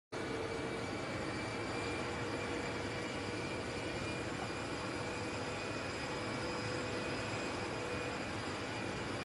Biden Uses Air Force One Short Stairs To Avoid Falling—Almost Falls
Hours after Axios reported that aides close to President Joe Biden are working to ensure he doesn't have a public fall again by deploying the short stairs, Biden nearly slipped down while deplaning Air Force One on Tuesday.